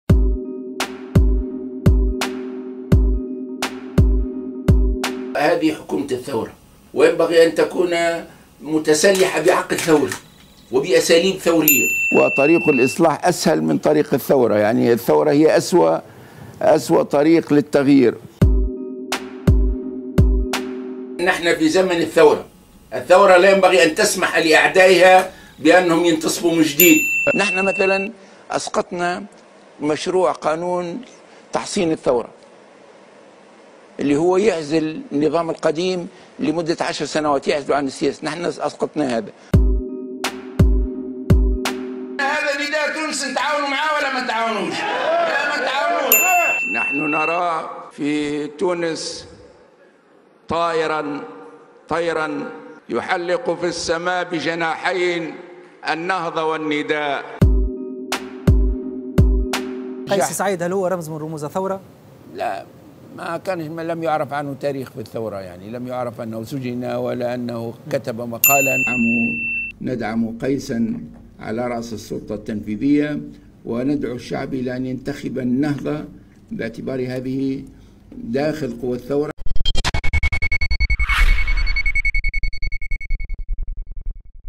غنوشي vs غنوشي
منذ عودته من منفاه في لندن سنة 2011، دأب زعيم حركة النهضة على مناقضة تصريحاته ومواقفه. بين الدعوة إلى كنس بقايا التجمع إلى رفض قانون تحصين الثورة، ومن معاداة نداء تونس إلى التحالف معه وصولا إلى إعتبار المرشح الرئاسي قيس سعيد ممثلا للخط الثوري بعد تصريح سابق انكر خلاله معرفة أي نشاط ثوري لسعيّد، تقلّبت مواقف الغنوشي دائما وفق مصالح حركة النهضة ومتغييرات المشهد السياسي.